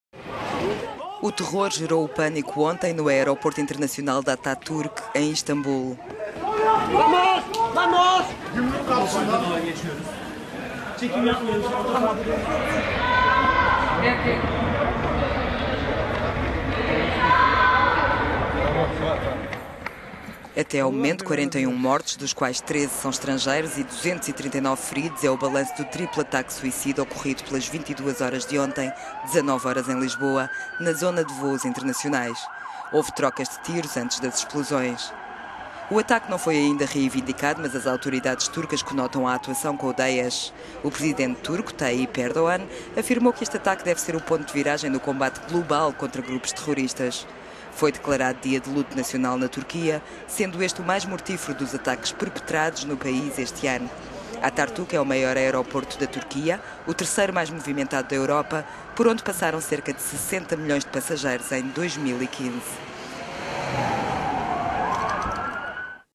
Terrorismo no aeroporto de Istambul: 41 mortos, 239 feridos
O terror gerou o pânico ontem no aeroporto internacional de Atatürk, em Istambul.

Até ao momento, 41 mortos, -dos quais 13 são estrangeiros,- e 239 feridos é o balanço do triplo ataque suicida ocorrido pelas 22 horas locais de ontem na zona de voos internacionais.
Houve trocas de tiros antes das explosões.

O ataque não foi ainda reivindicado mas as autoridades turcas conotam a actuação com o Daesh. O presidente turco, Tayyp Erdogan, afirmou que este ataque deve ser o ponto de viragem no co…